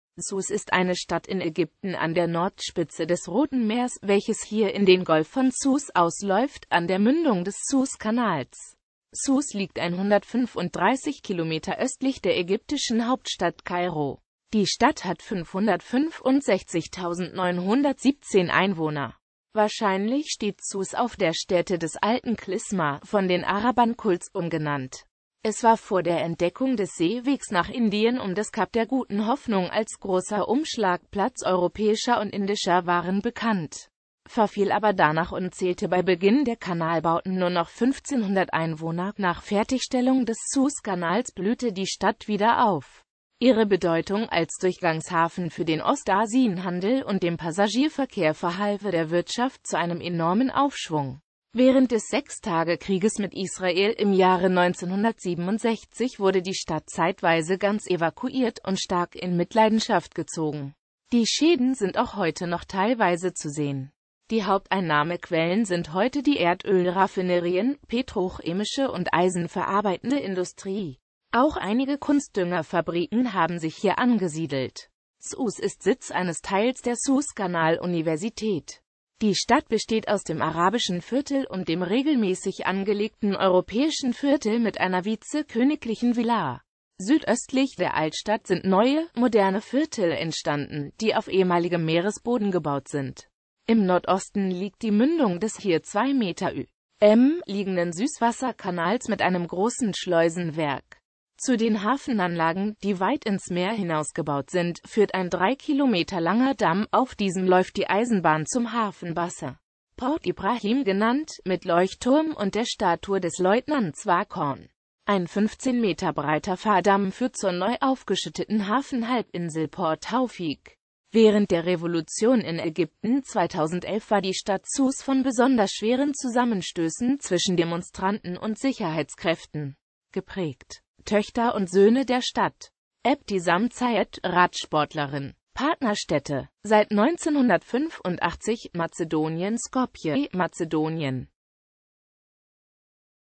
Sues
Vielen Dank für Ihre Unterstützung:

Sues ist eine Stadt in Ägypten an der Nordspitze des Roten Meers, welches hier in den Golf von Sues ausläuft, an der Mündung des Sueskanals.Sues liegt 135 km östlich der ägyptischen Hauptstadt Kairo.Die Stadt hat 565.917 Einwohner .Wahrscheinlich steht Sues auf der Stätte des alten Klysma, von den Arabern Qulzum genannt.

Bild-Copyright-Informationen
Urheber Info: NordNordWest